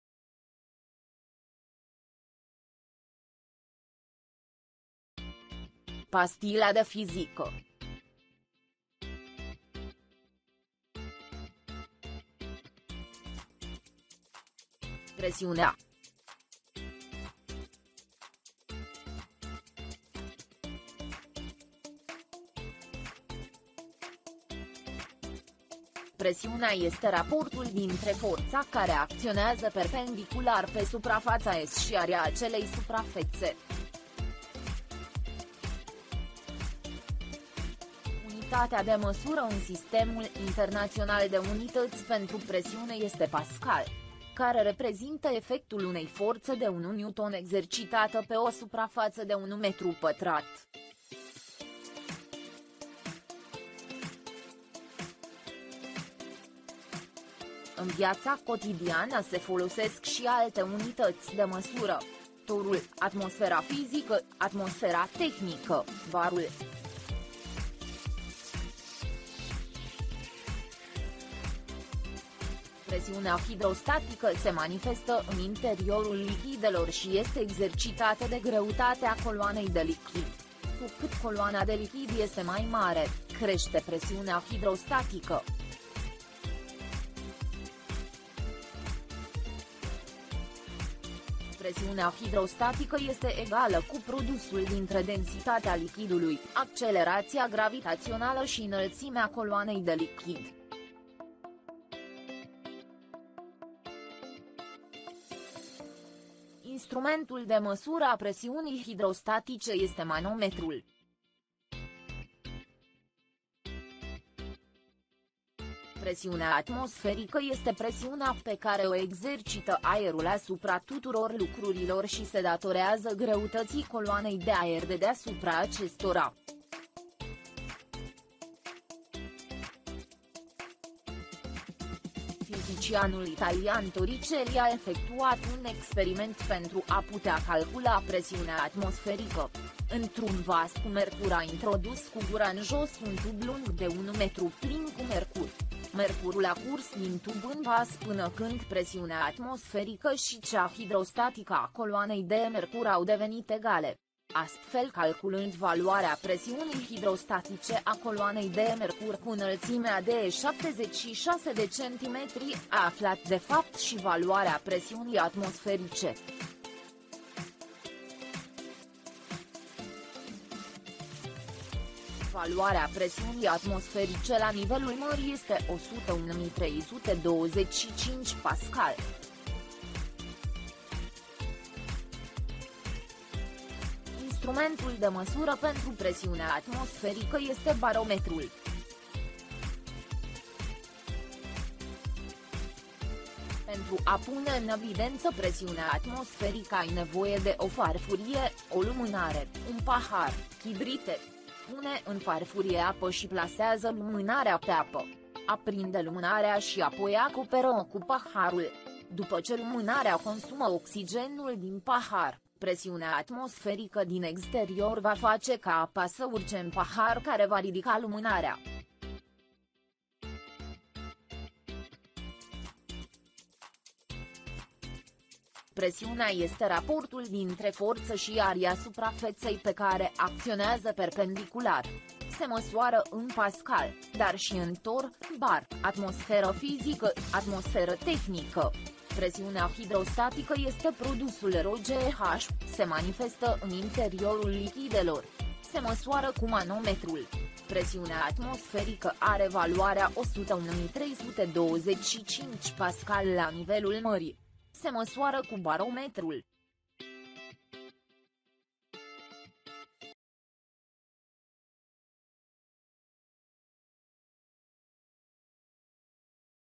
Presiunea
Resursa educationala “Pastila de fizica” se doreste a fi un film scurt de 2-5 minute si care sintetizeaza elementele de baza necesare intelegerii notiunii abordate in video:
 definitii
 exemple
 formule
 imagini sugestive pentru intelegerea temei
 animatii sugestive pentru intelegerea temei
 aplicatie practica: experiment pentru evidentierea presiunii atmosferice
 infografic recapitulativ
Profesorul poate utiliza resursa ca element de fixare a cunostintelor in lectia de predare a notiunii de presiune, respective pentru diferitele tipuri de presiune sau intr-o lectie de recapitulare a capitolului “Fenomene mecanice. Statica fluidelor”.
Resursa este un material video, astfel profesorul sau elevul pot pune pe pauza video-ul atat timp cat considera necesar pentru a relua explicatiile.
Resursa a fost conceputa in asa fel incat orice elev sa o poata parcurge si singur acasa sau la scoala, dar aceasta poate fi parcursa si frontal la clasa sau in sistem online.
Profesorul poate oferi explicatii suplimentare elevilor care au nevoie de acestea sau pot permite altor elevi de a explica peer to peer colegilor informatiile, formandu-si astfel limbajul stiintific necesar.

Autor: Moise Rodica